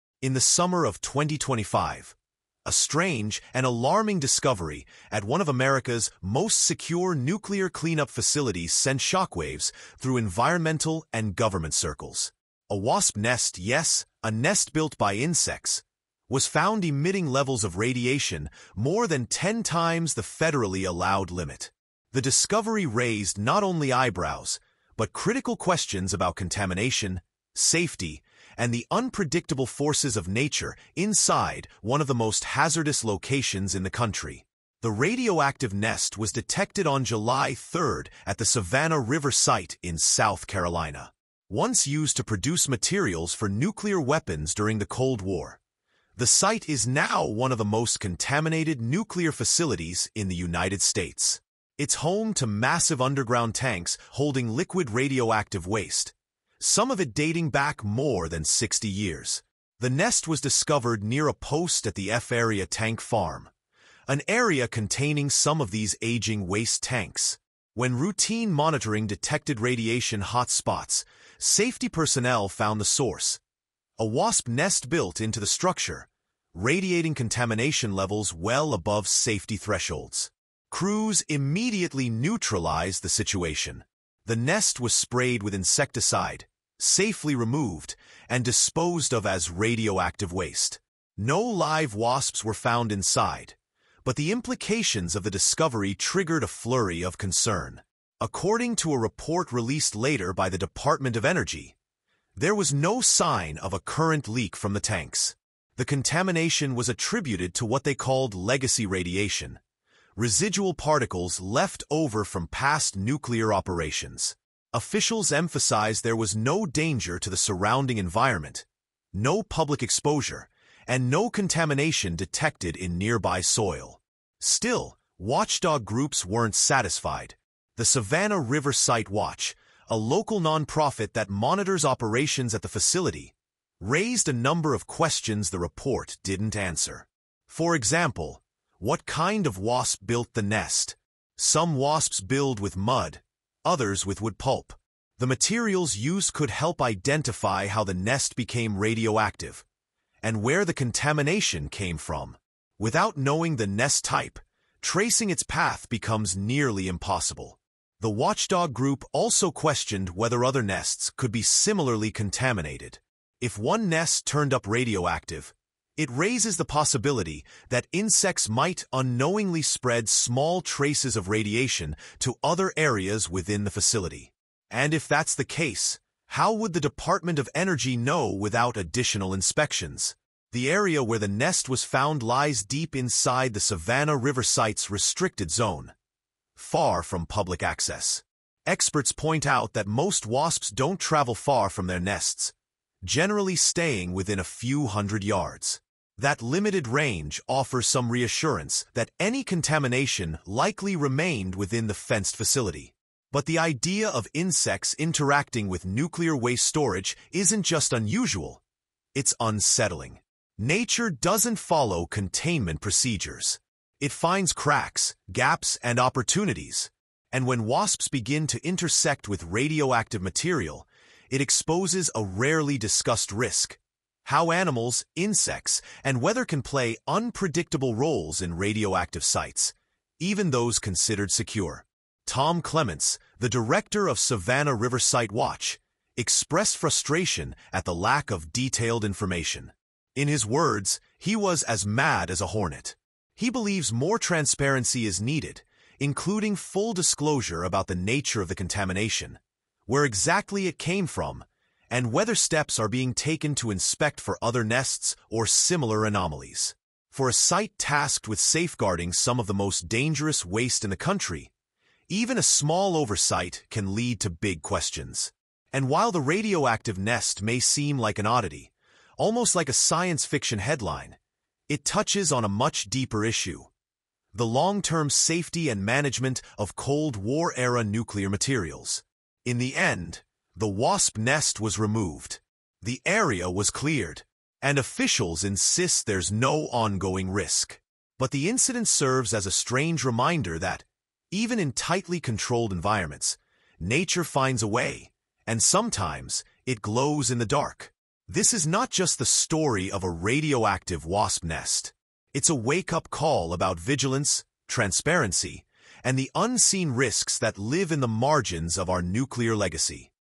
Radioactive Wasp Nest Discovered at U.S. Nuclear Site | Savannah River Nuclear Mystery Explained
A radioactive wasp nest was found at the Savannah River Site in South Carolina, one of America's most hazardous nuclear cleanup zones. The nest was emitting radiation levels over ten times the legal safety limit, sparking concern and questions about contamination, insect activity, and nuclear waste management. In this video, we explain what happened, what the Department of Energy reported, and why watchdog groups aren’t satisfied with the answers. Learn how a wasp nest became part of a nuclear safety investigation—and what it could mean for the future of environmental oversight at radioactive sites.

In the summer of 2025, a strange and alarming discovery at one of America’s most secure nuclear cleanup facilities sent shockwaves through environmental and government circles. A wasp nest—yes, a nest built by insects—was found emitting levels of radiation more than ten times the federally allowed limit. The discovery raised not only eyebrows, but critical questions about contamination, safety, and the unpredictable forces of nature inside one of the most hazardous locations in the country.

The radioactive nest was detected on July 3rd at the Savannah River Site in South Carolina. Once used to produce materials for nuclear weapons during the Cold War, the site is now one of the most contaminated nuclear facilities in the United States. It’s home to massive underground tanks holding liquid radioactive waste—some of it dating back more than 60 years.

The nest was discovered near a post at the F-Area Tank Farm, an area containing some of these aging waste tanks. When routine monitoring detected radiation hot spots, safety personnel found the source: a wasp nest built into the structure, radiating contamination levels well above safety thresholds.

Crews immediately neutralized the situation. The nest was sprayed with insecticide, safely removed, and disposed of as radioactive waste. No live wasps were found inside, but the implications of the discovery triggered a flurry of concern.

According to a report released later by the Department of Energy, there was no sign of a current leak from the tanks. The contamination was attributed to what they called legacy radiation—residual particles left over from past nuclear operations. Officials emphasized there was no danger to the surrounding environment, no public exposure, and no contamination detected in nearby soil.

Still, watchdog groups weren’t satisfied. The Savannah River Site Watch, a local nonprofit that monitors operations at the facility, raised a number of questions the report didn’t answer.

For example, what kind of wasp built the nest? Some wasps build with mud, others with wood pulp. The materials used could help identify how the nest became radioactive—and where the contamination came from. Without knowing the nest type, tracing its path becomes nearly impossible.

The watchdog group also questioned whether other nests could be similarly contaminated. If one nest turned up radioactive, it raises the possibility that insects might unknowingly spread small traces of radiation to other areas within the facility. And if that's the case, how would the Department of Energy know without additional inspections?

The area where the nest was found lies deep inside the Savannah River Site’s restricted zone—far from public access. Experts point out that most wasps don’t travel far from their nests, generally staying within a few hundred yards. That limited range offers some reassurance that any contamination likely remained within the fenced facility.

But the idea of insects interacting with nuclear waste storage isn’t just unusual—it’s unsettling. Nature doesn’t follow containment procedures. It finds cracks, gaps, and opportunities. And when wasps begin to intersect with radioactive material, it exposes a rarely discussed risk—how animals, insects, and weather can play unpredictable roles in radioactive sites, even those considered secure.

Tom Clements, the director of Savannah River Site Watch, expressed frustration at the lack of detailed information. In his words, he was “as mad as a hornet.” He believes more transparency is needed, including full disclosure about the nature of the contamination, where exactly it came from, and whether steps are being taken to inspect for other nests or similar anomalies.